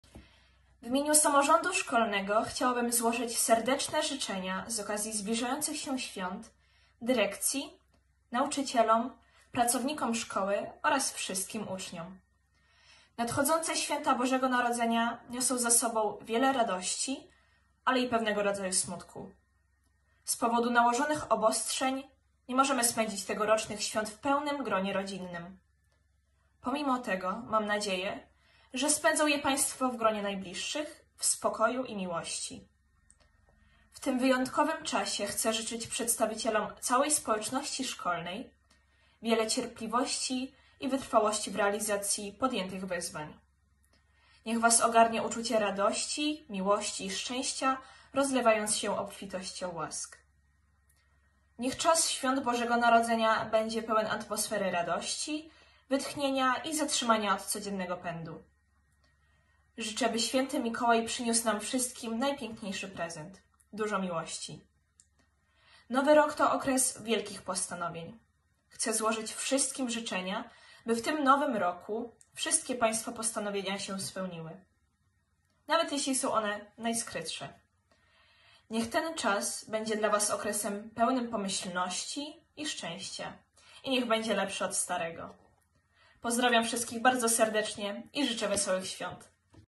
Życzenia Świąteczne przewodniczącej Samorządu Uczniowskiego Julii Drożdż
Życzenia Świąteczne przewodniczącej Samorządu Uczniowskiego w I LO w Radzyniu Podlaskim Julii Drożdż